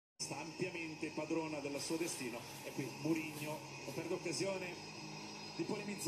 Mourinho chambre les supporter des la Juve suite a leurs victoire 1-2
Mourinho chambre les supporter des la Juve suite a leurs victoire 1-2 en Ligue des Champions